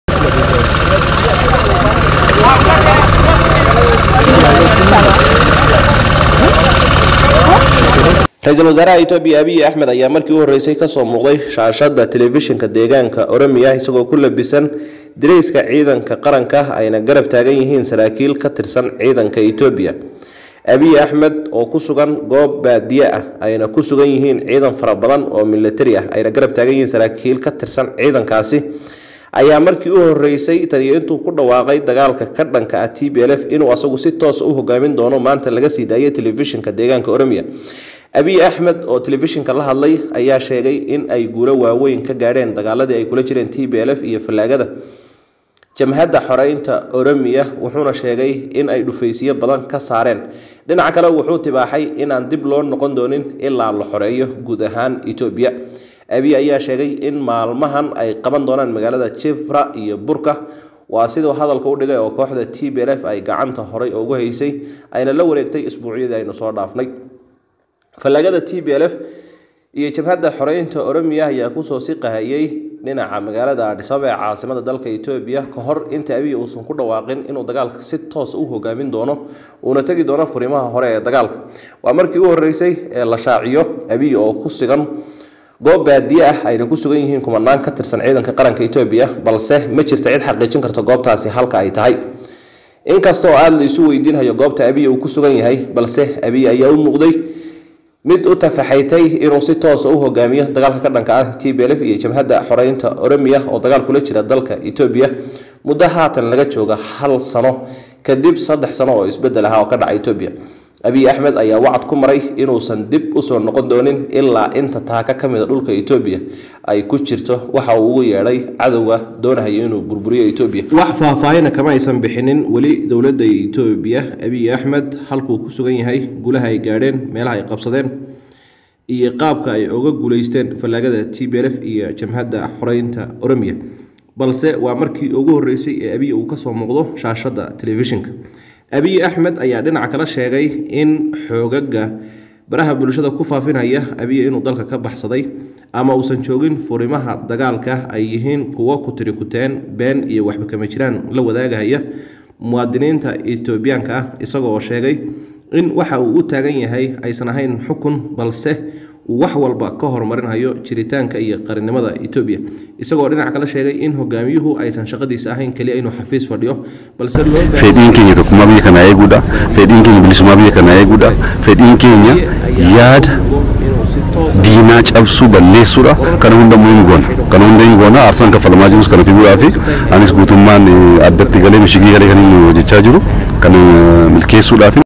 DEG-DEG: Abiy Axmed ayaa ka soo muuqda Furinta Hore ee Daagalka isagoo ku labisan Dariska Ciidanka..
Abiy